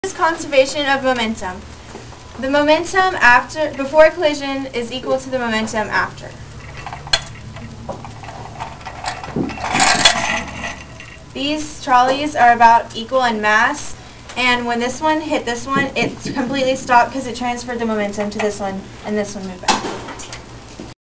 Conservation of Momentum
A student demonstrates the conservation of momentum during an elastic collision.